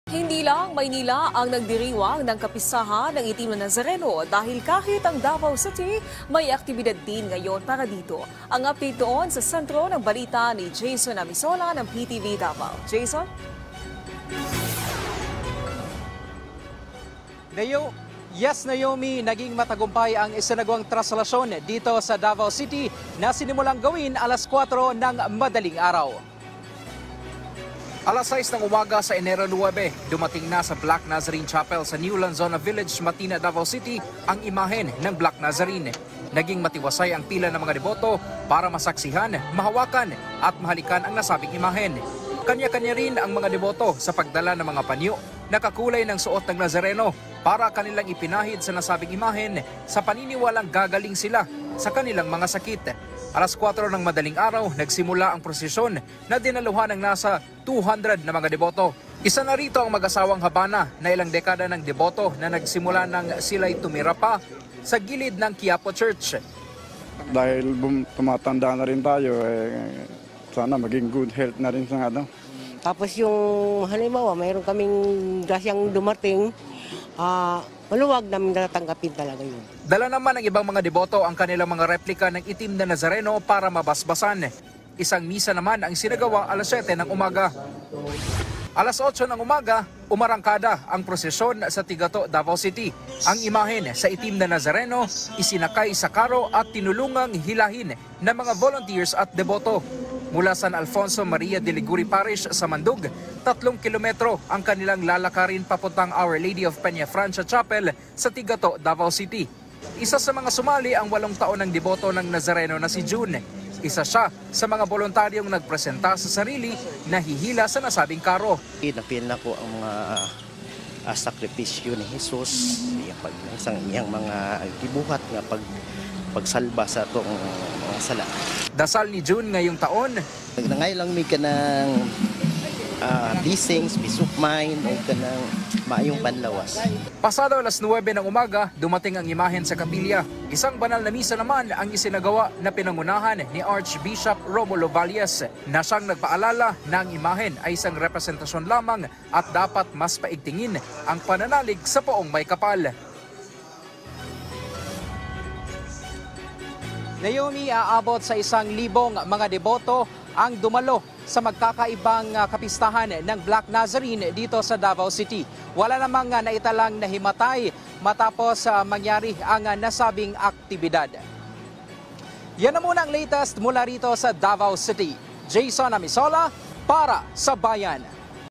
Traslacion2024 ng Poong Itim na Nazareno, isinagawa din sa Davao City;
Nasa 200 mga deboto, nakiisa sa prusisyon